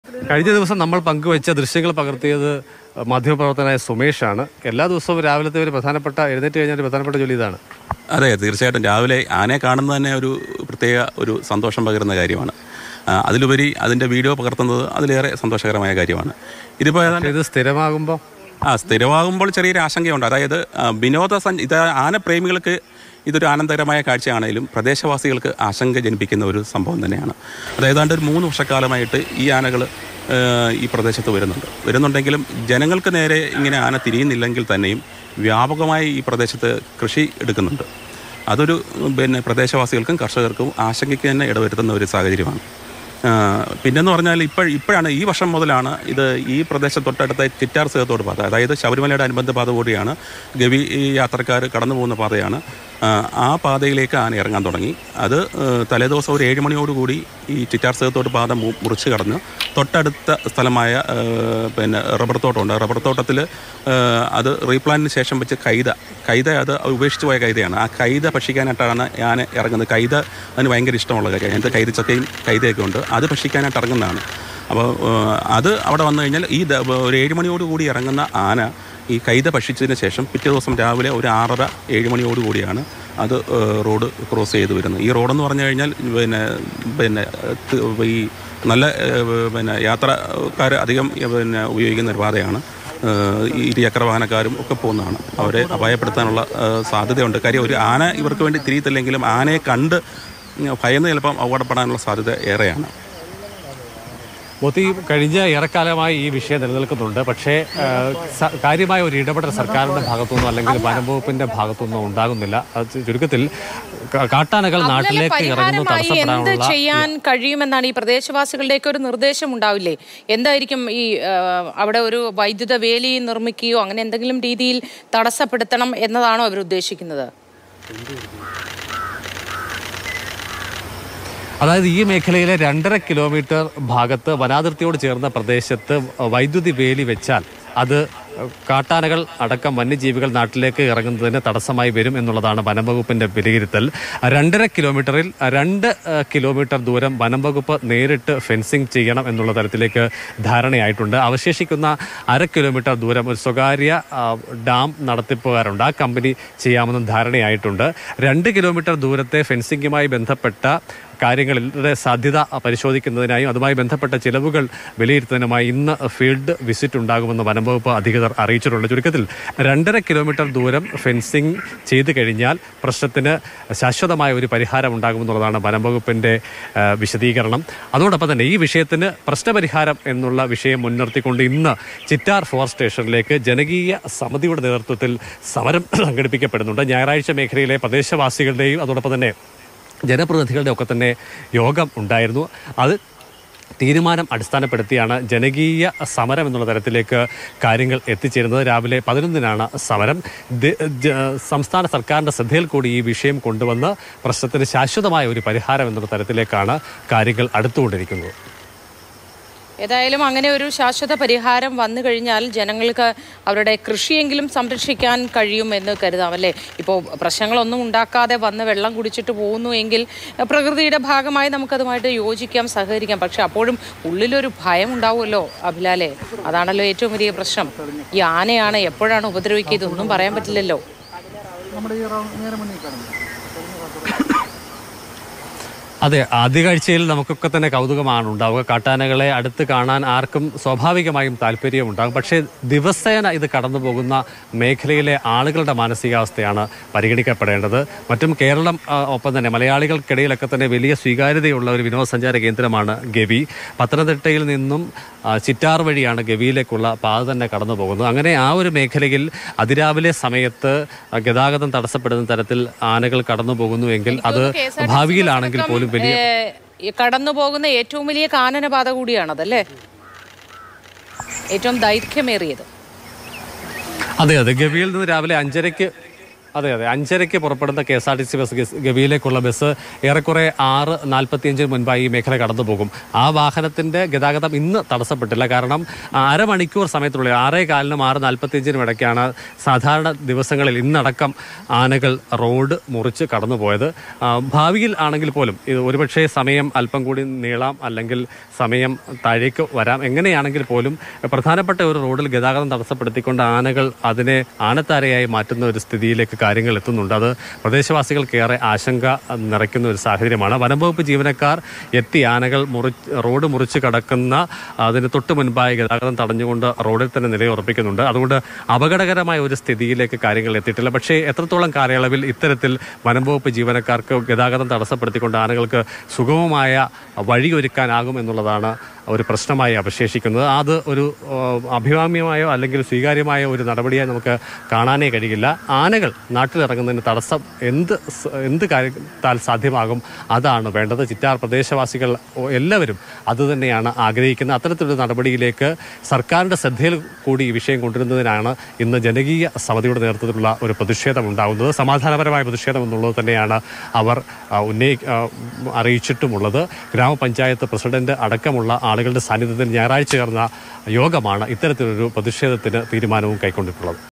'ആളുകളെ ആക്രമിക്കുന്നില്ലെങ്കിലും വ്യാപക കൃഷിനാശം ഉണ്ടാക്കുന്നുണ്ട്; ഭീതിപരത്തി ആനകൾ | Wild Elephant
ആനപ്രേമികൾക്ക് ആനന്ദമാണ് പക്ഷേ നാട്ടുകാർക്ക് ആശങ്കയാണ്; ചിറ്റാരിൽ ഭീതിപരത്തി ആനശല്യം

മാറുന്ന ലോകത്ത് കൂടുതൽ മാറ്റങ്ങളോടെ. പുതിയ വേഗത്തിൽ. പുതിയ ലുക്കിൽ.
മാതൃഭൂമി ന്യൂസ്.

Happy viewing!